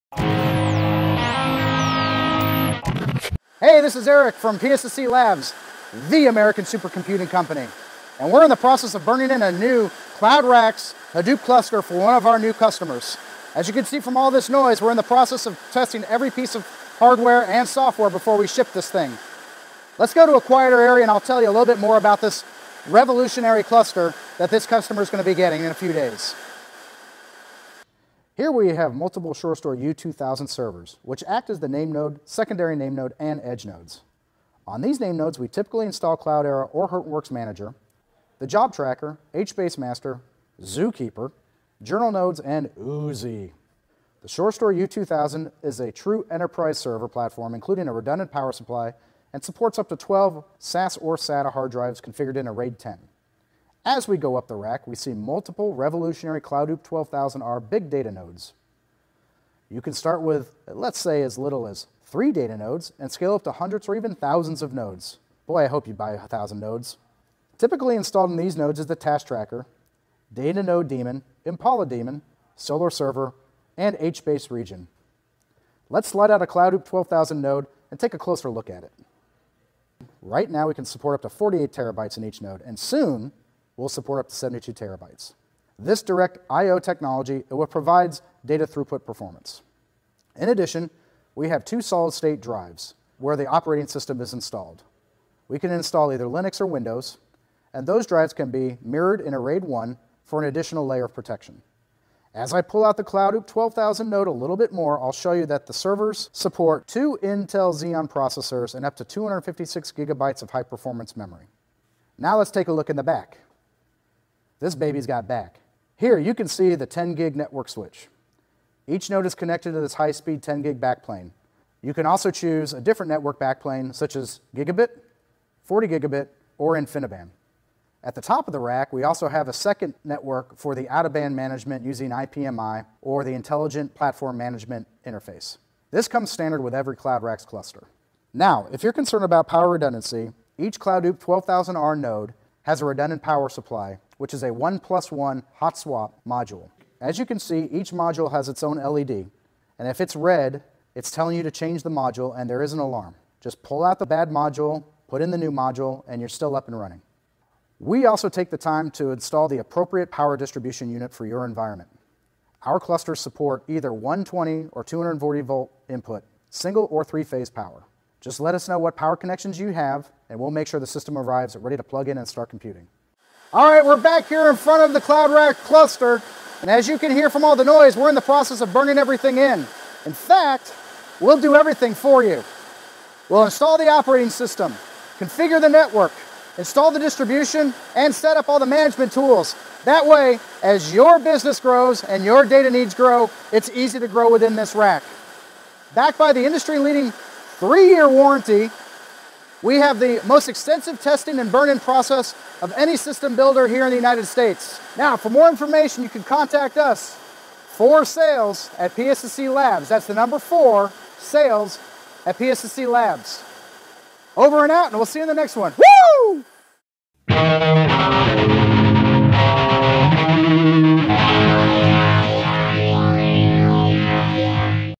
CloudRax Turnkey Hadoop Cluster
Turn-key cluster for Big / Data Hadoop.  CloudRax includes all necessary hardware, software & networking for your Enterprise Data Hub or Data Lake.  Certified compatible with Cloudera, Hortonworks, & MapR.   CloudRax clusters can start with a small number of nodes & scale to thousands of nodes.  Custom configure CloudRax for your exact needs / budget.